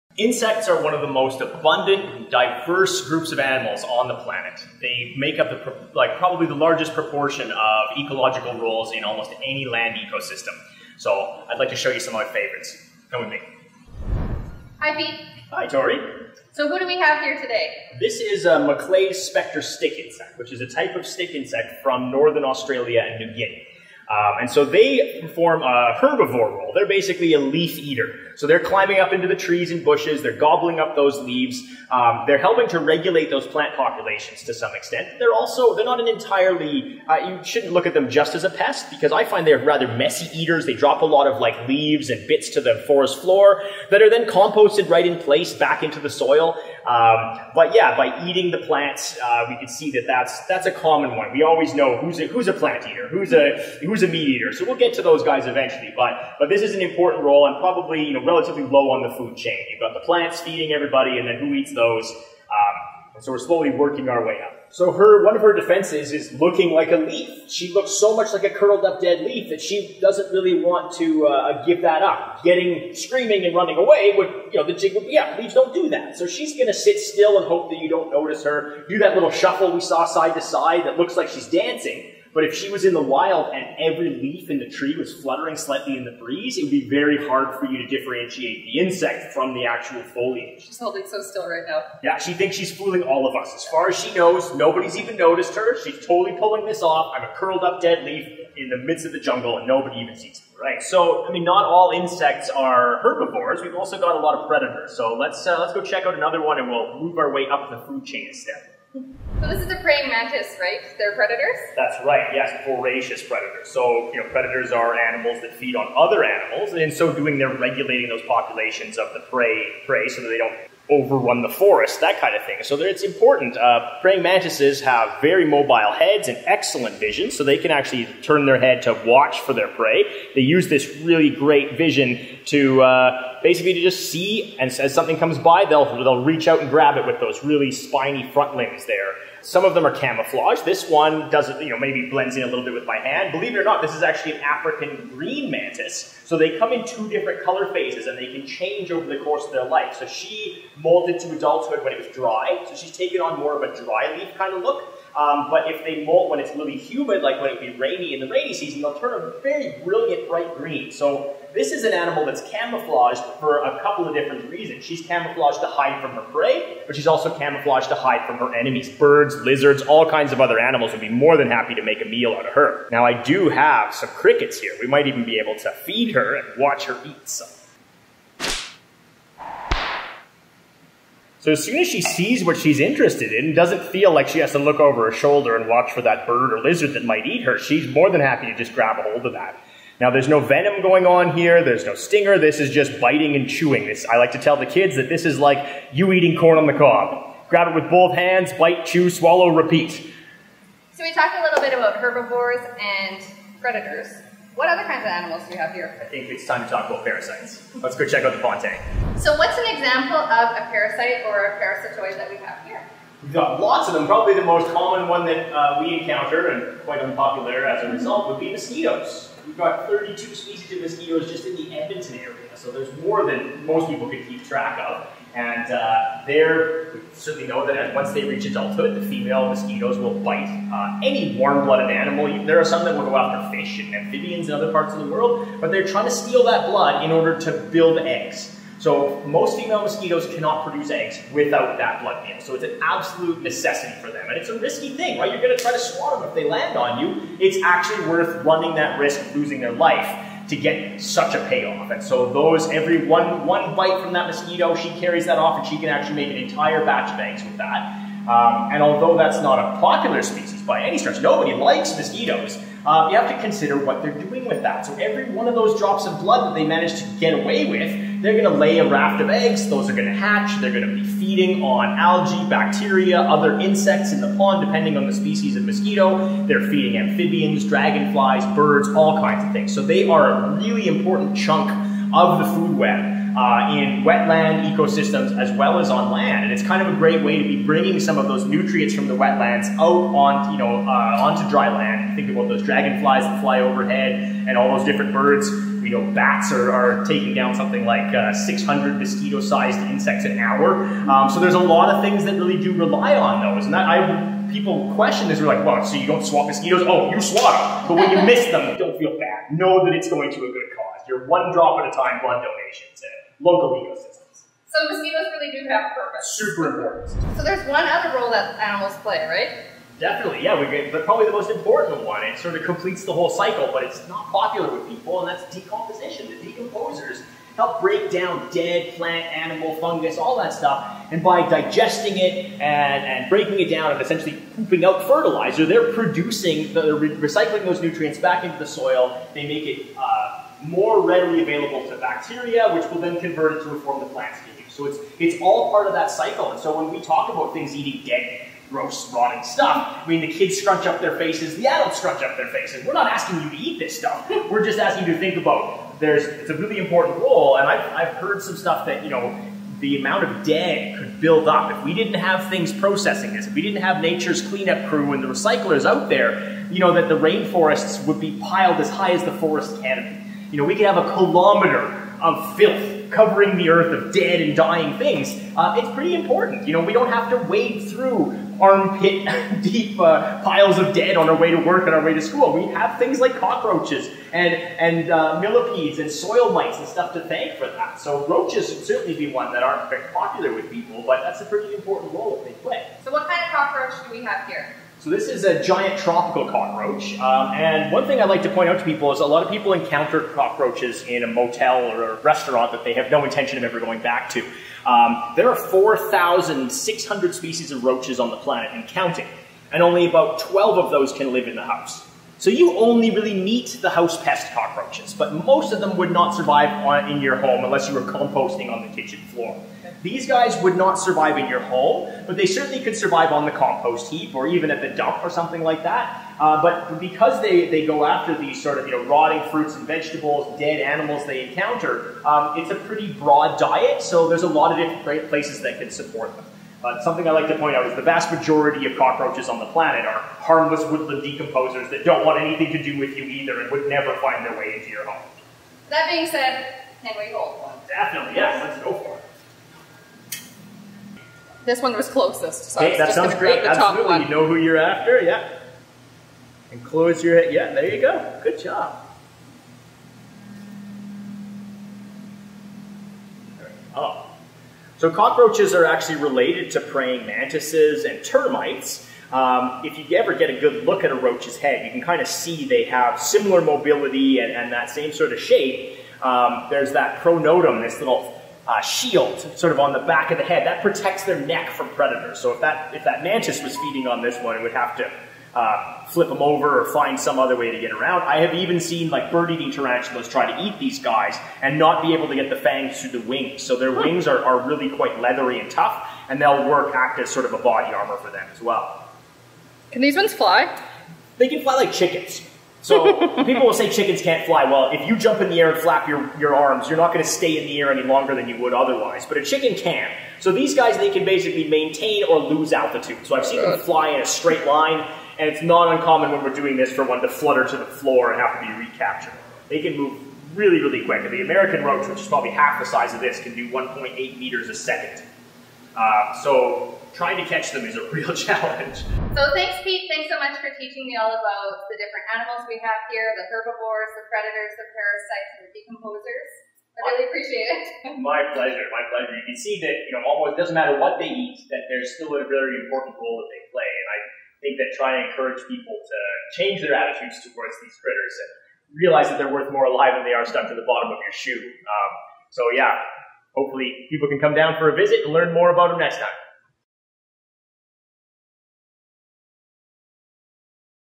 Meet our bugs! Macleay's spectre stick insect, African green mantis, Giant tropical cockroach
Insects are one of the most diverse groups of animals, with a variety of ecological roles. Come meet these four insects who fulfill the roles of herbivore, predator, parasite and composter.